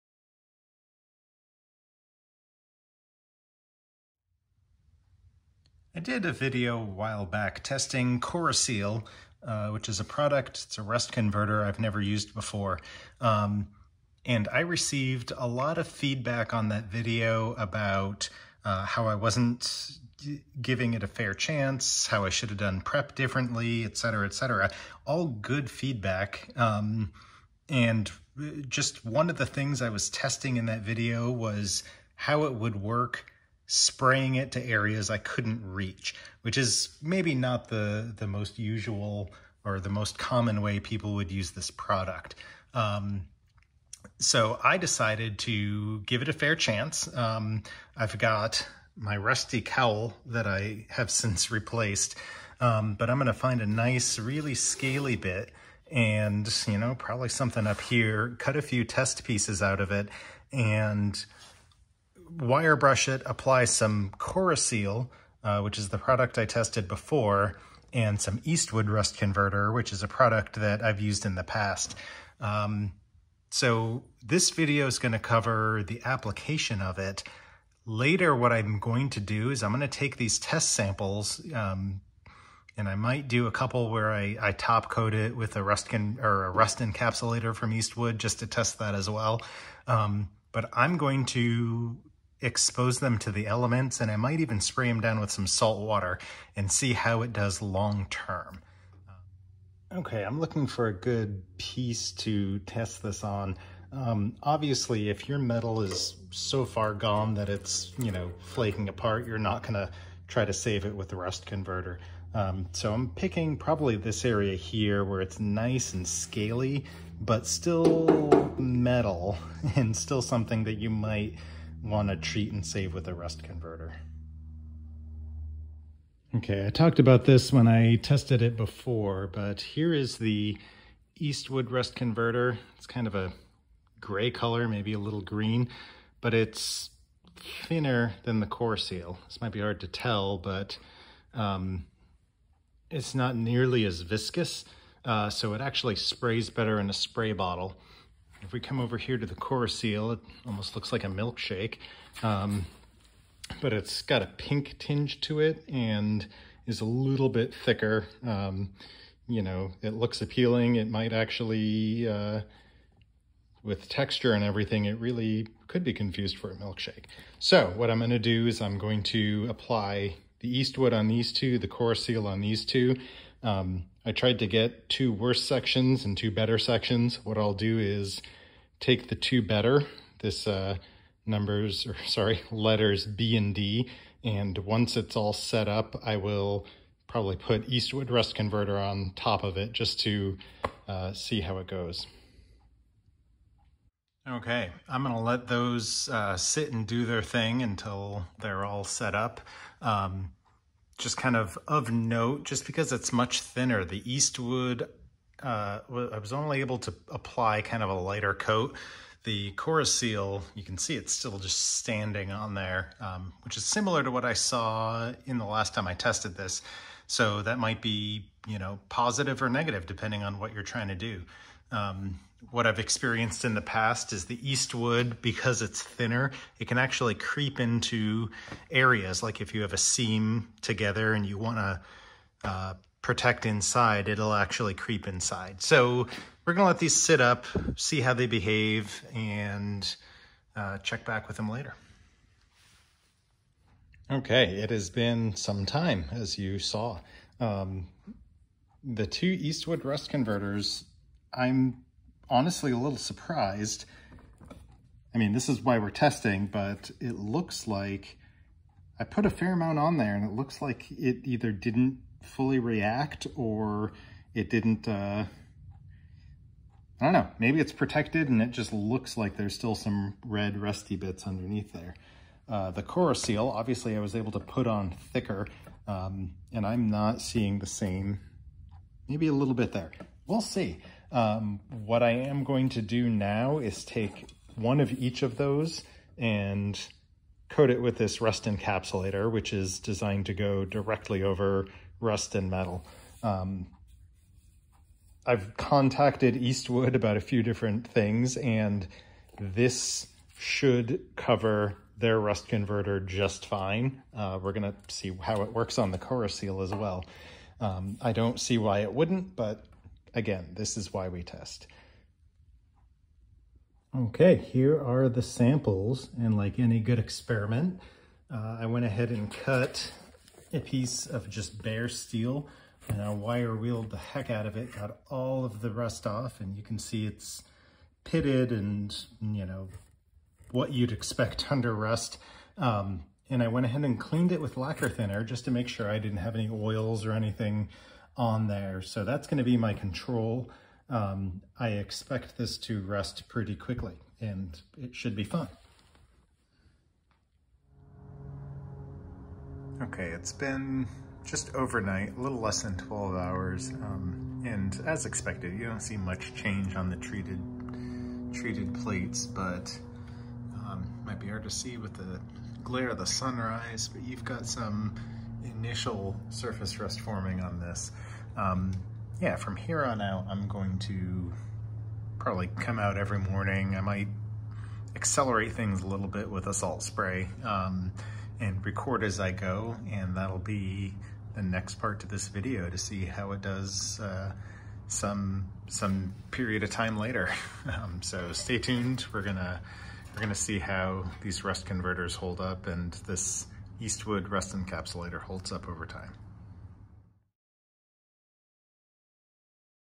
Corroseal vs. Eastwood Rust Converter Challenge Part 1: Application
Let the great experiment begin! I wanted to know what works for protecting hard to reach steel from rust, so I’m testing Corroseal and Eastwood rust converters on some very rusty metal. I over-coated two of the samples with Eastwood Rust Encapsulator. The plan is to leave the samples exposed to the elements and some salt spray to see how well they hold up both unprotected and top-coated. Maybe not the most thorough test, but good enough to see how the rust converters fare if untreated!

Eastwood Rust Converter:

It was noted by a viewer that the Eastwood calls for a second coat after 20 minutes, which I didn’t do. I’m fairly certain this wasn’t the case ~10 years ago when I first used this product, but sure enough it does now!

I plan to post updates and results in future videos.